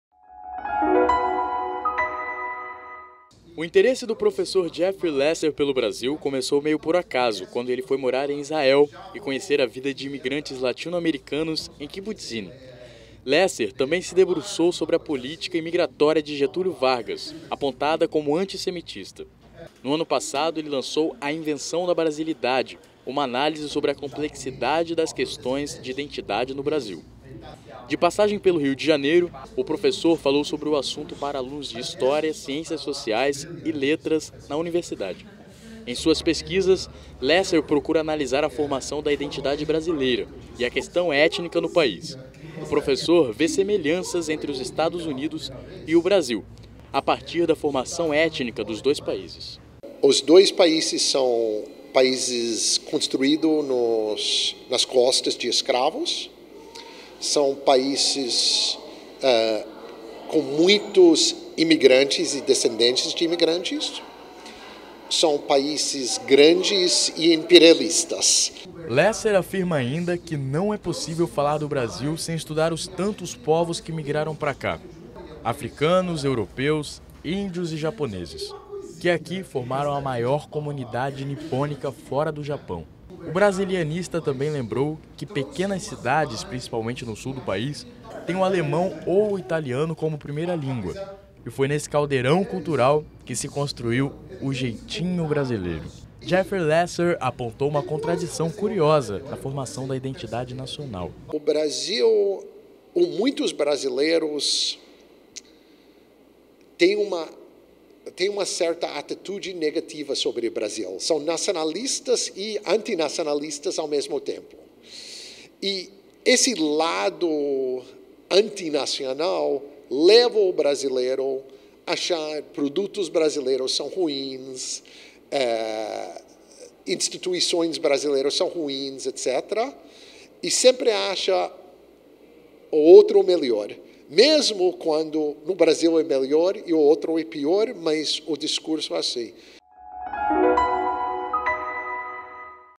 TV PUC-Rio: Jeffrey Lesser analisa a complexa formação da identidade brasileira
A brasilidade não foi construída somente a partir do tripé negros, índios e portugueses, mas também é resultado da complexa negociação entre esses grupos e outros, como japoneses, judeus, árabes e outros europeus. Em palestra na PUC-Rio, Jeffrey Lesser falou sobre identidade e questões étnicas no Brasil.